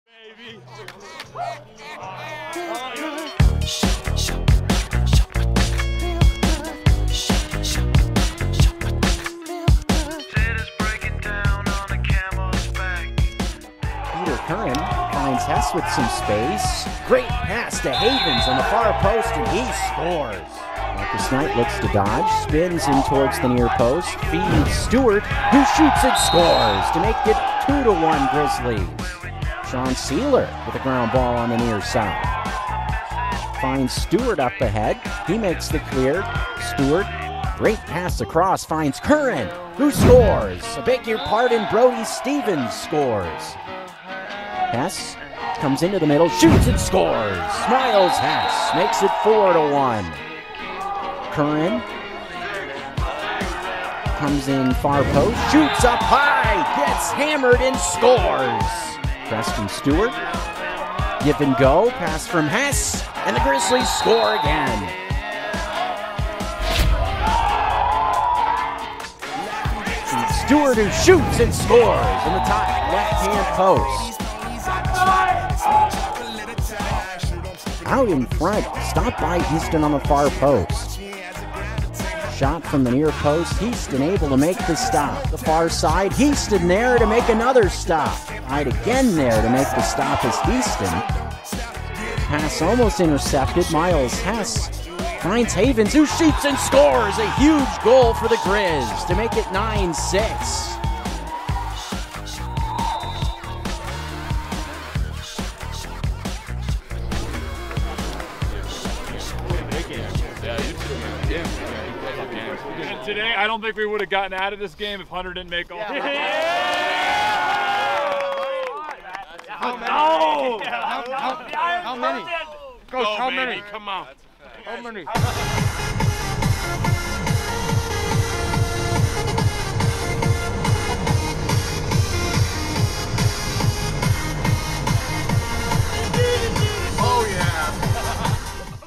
Montana vs. Minnesota-Duluth Lacrosse Highlights (MCLA)
Full Highlights from our 9-7 win over @umdlacrosse. Dogs played us tough, but shoutout to Preston “Say My Name” Stewart and our goalie Hunter “The Iron Curtain” Heaston for red letter days.